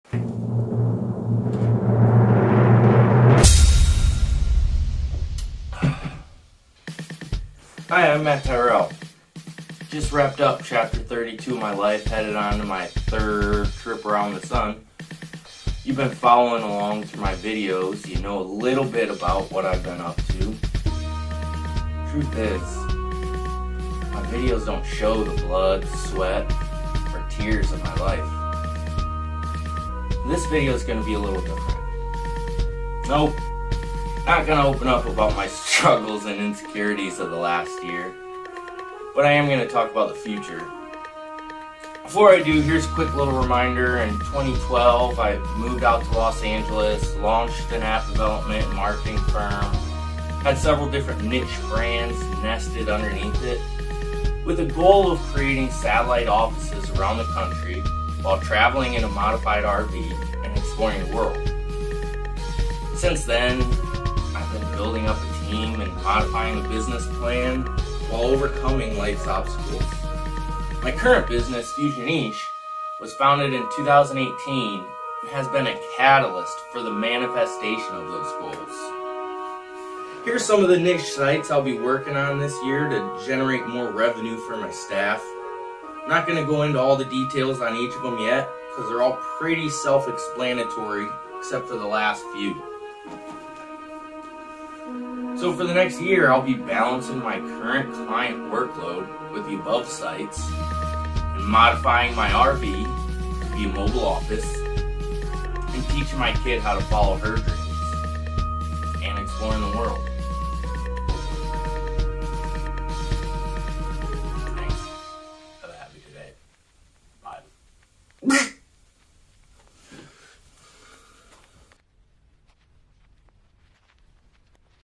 Have A Happy Today: 33rd Anniversary Video
Hi, I’m Matt Tyrrell. Just wrapped up chapter 32 of my life, and headed into my 33rd trip around the sun. If you’ve been following along through my videos, you know a little bit about what I’ve been up to. Truth is, my videos don’t show the blood, sweat or tears of my life. This video is going to be a little different. Nope, not going to open up about my struggles and insecurities of the last year, but I’m going to talk about the future.

Before I do, here’s a quick little reminder, that in 2012 I moved to Los Angeles and launched an app development & marketing firm that had several different niche brands nested underneath it, with a goal of creating satellite offices around the country, while traveling in a modified RV and exploring the world. Since then, I have been building up a team and modifying the business plan while overcoming life’s obstacles. My current business, Fusioniche, was founded in 2018 and has been a catalyst for the manifestation of my goals.

These are some of the niche sites I’ll be working on this year to generate more revenue for my staff. I’m not going to go into all the details on each of them yet, because they are all pretty self explanatory except the last few.

StartAnAdvertisingAgency.com
AdventureMarketingGuide.com
ArtistMarketingGuide.com
AttorneyMarketingGuide.com
BnbMarketingGuide.com
ChurchMarketingGuide.com
CommunityMarketingGuide.com
ConstructionMarketingGuide.com
ElectricianMarketingGuide.com
InsuranceMarketingGuide.com
LandscaperMarketingGuide.com
MedicalMarketingGuide.com
MultilevelMarketingGuide.com
PlumberMarketingGuide.com
RealtyMarketingGuide.com
RestaurantMarketingGuide.com

HomeConstructionGuide.com
CustodyGuide.org
TheOriginalSocialNetwork.com
TheSuperDads.org
CallMeForAGoodTime.com
HaveAHappyToday.com

So, for the next year, I’ll be balancing my current client workload with the above sites, and modifying my RV to be a mobile office, and teaching my kid how to follow her dreams, and exploring the world. Hopefully.